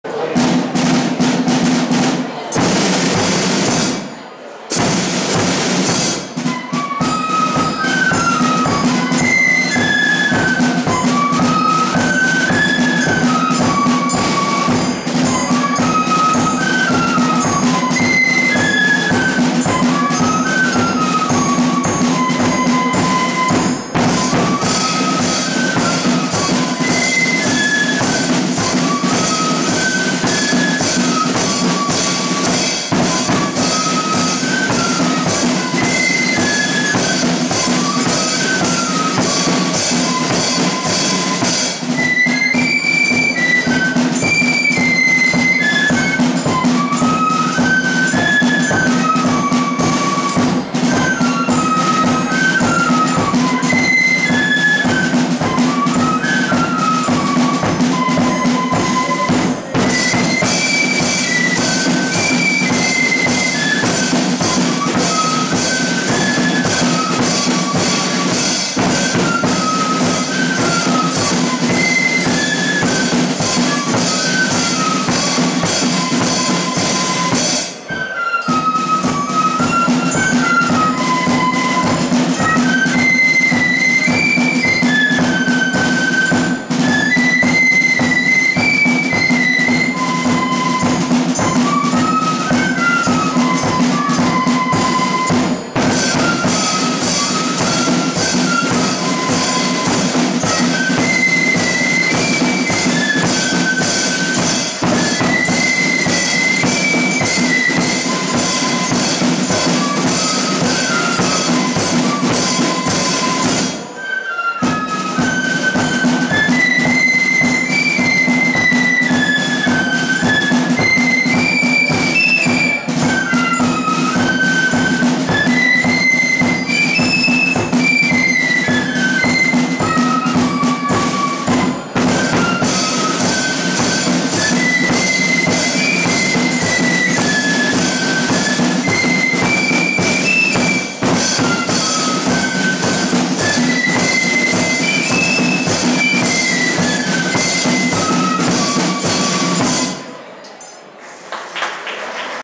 Ballymacarrett Defenders 2 @ Festival of Flute Bands 2023
Ballymacarrett Defenders playing at Lisburn Young Defenders Festival of Flute Bands 2023